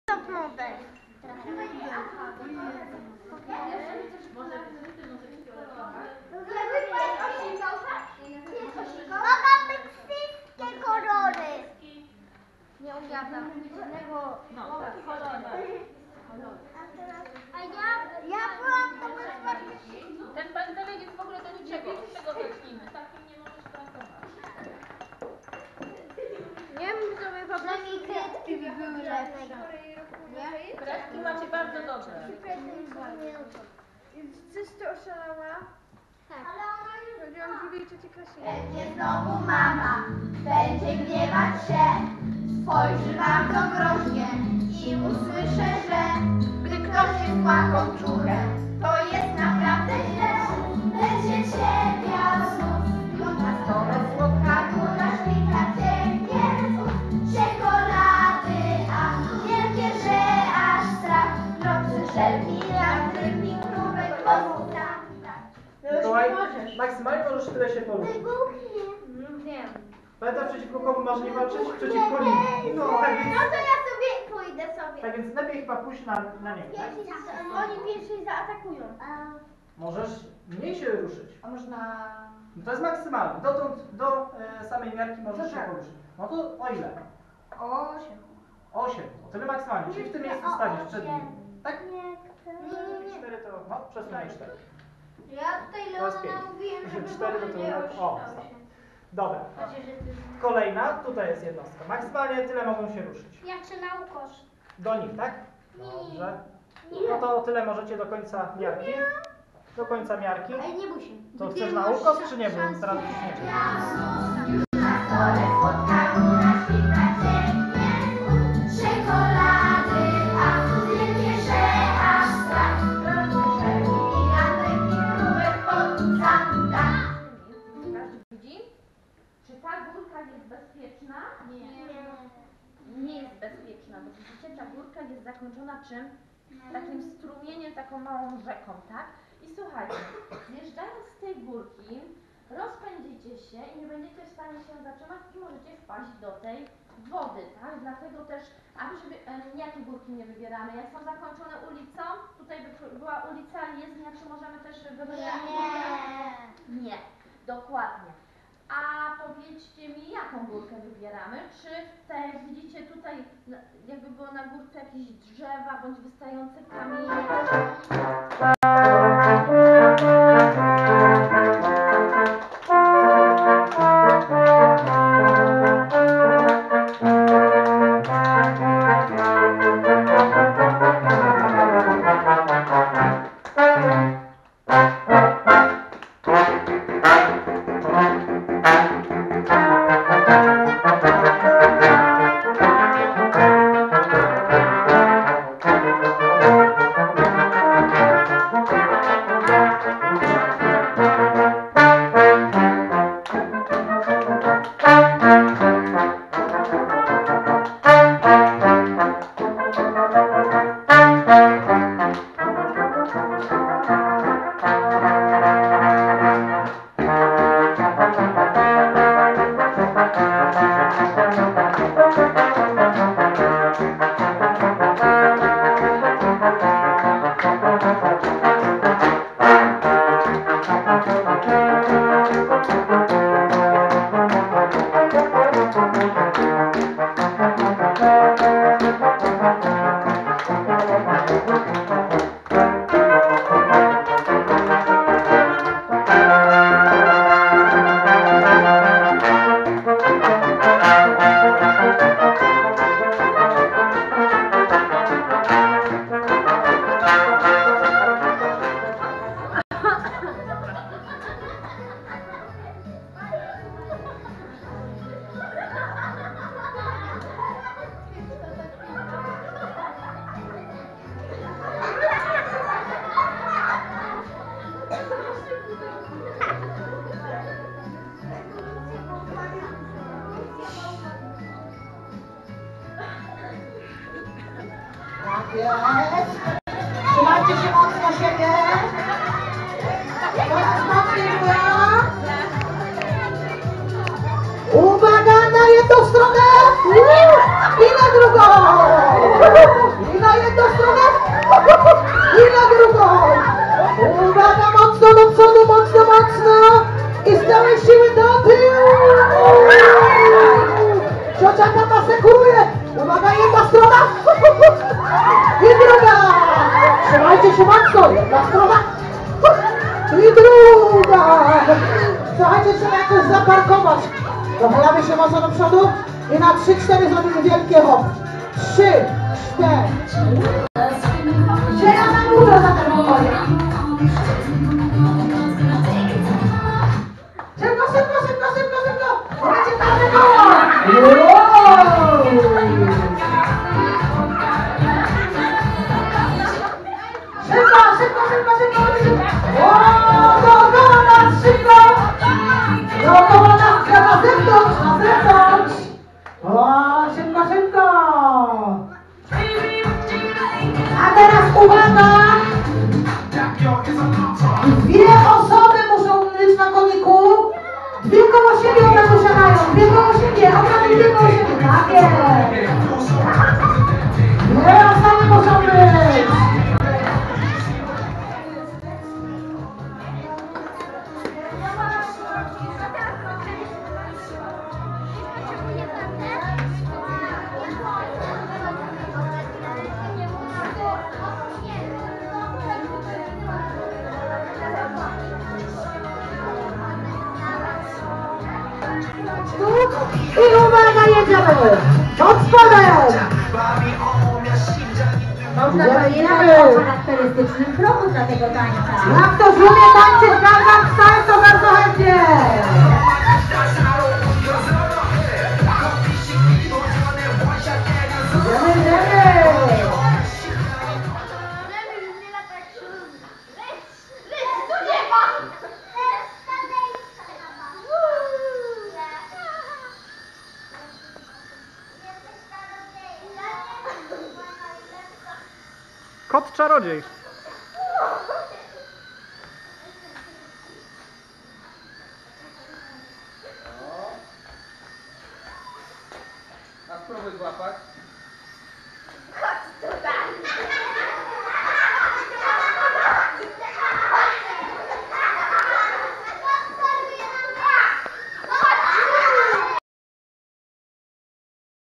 Ferie dla dzieci w Klubie Raszyn Zima 2017 - II tydzień
Fragmenty zajęć dla dzieci jakie zorganizowane zostały w Klubie Raszyn Spółdzielni Mieszkaniowej Grunwald w Poznaniu podczas drugiego tygodnia akcji zimowej "Ferie w klubie".
Warsztaty plastyczne, Studio Piosenki dla dziewczynek, Gry bitewne dla chłopców, Spotkanie ze Strażniczkami Miejskimi, Warsztaty muzyczne, Dobry Teatr przedstawienie i warsztaty integracyjne